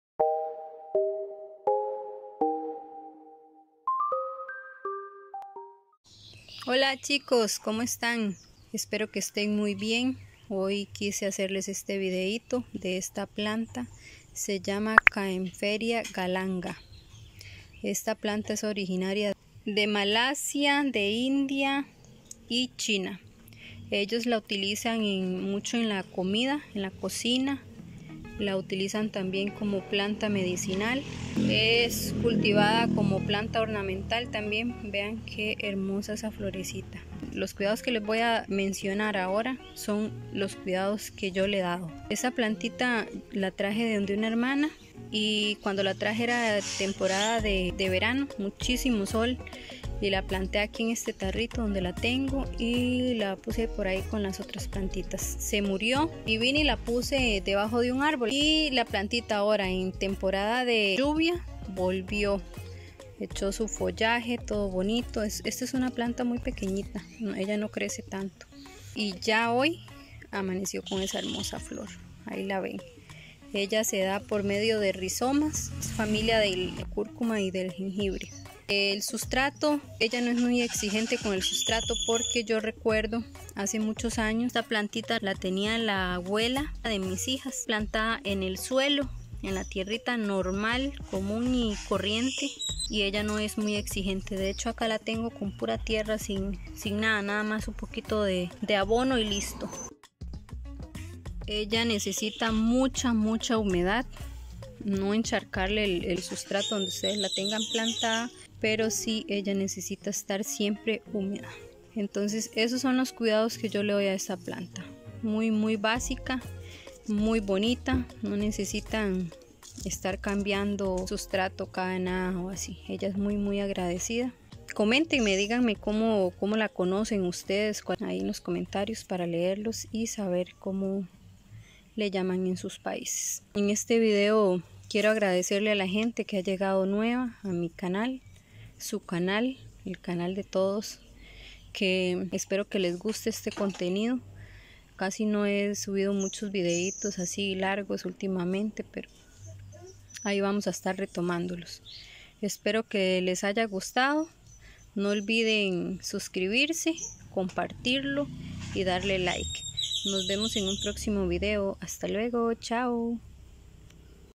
¿Cómo cuidar Kaempferia Galanga? | Cuidados, tips y más
En este video les mostraré los cuidados que le doy a esta plantita!
Su nombre es Kaempferia Galanga.